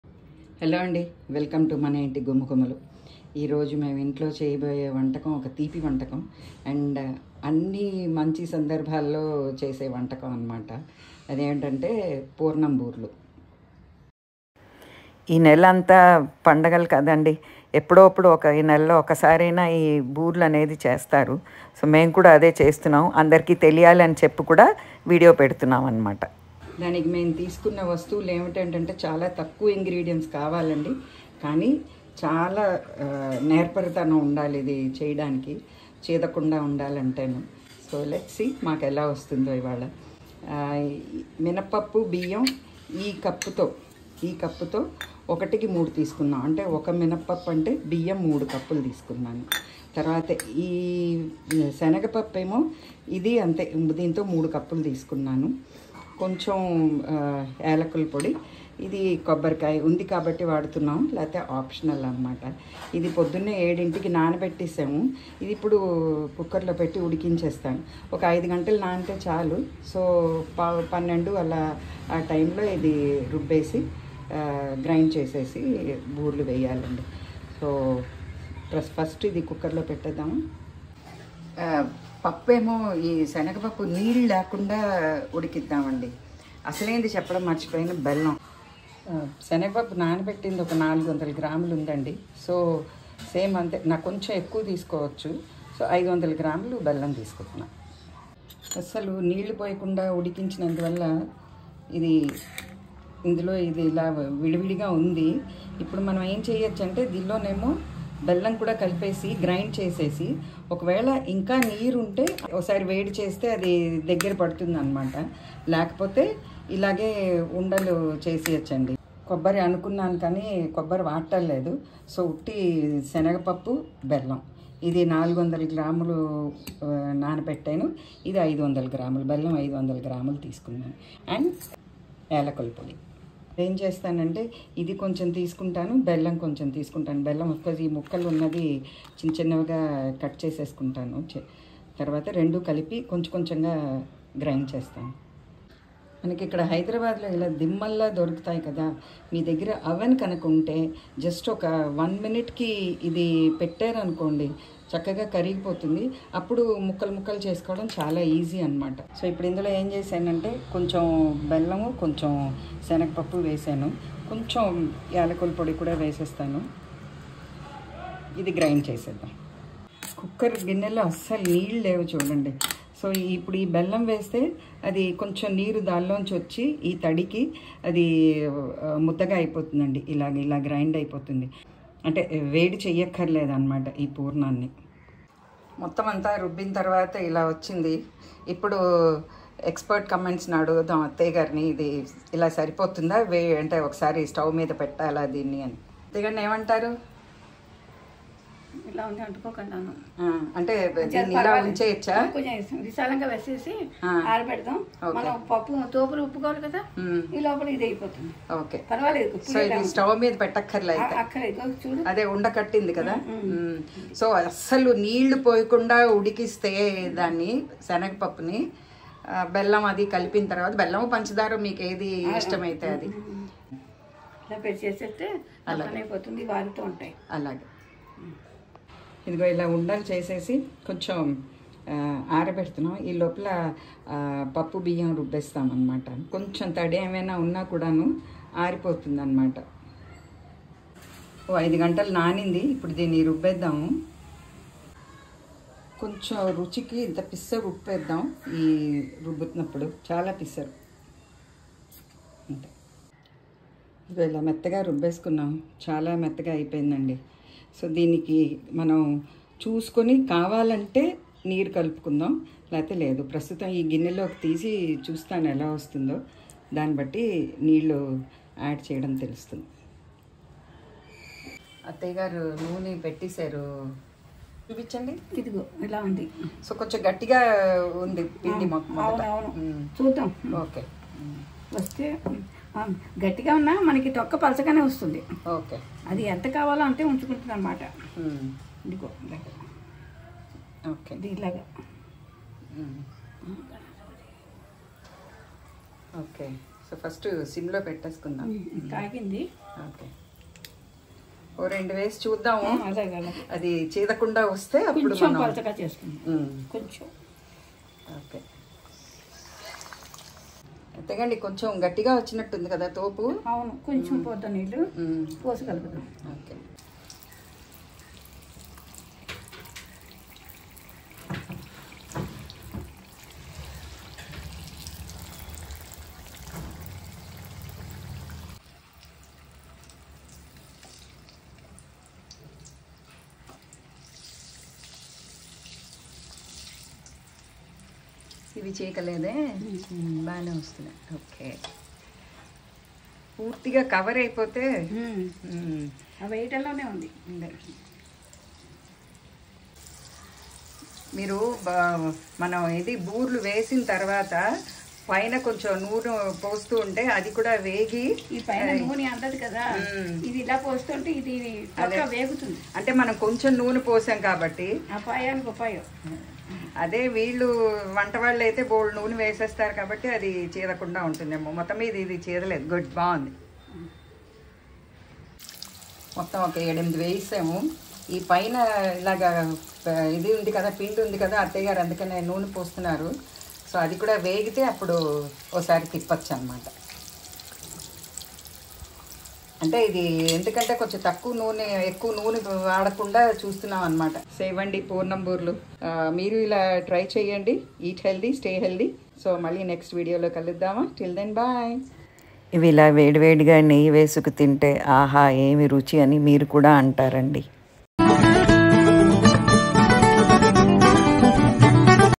Poornam Boorelu - Very , very tasty , delicious and not so Easy Recipe 😊
Ingredients : senagapappu , Minapappu, biyyam, elakkaya powder, noone and koncham uppu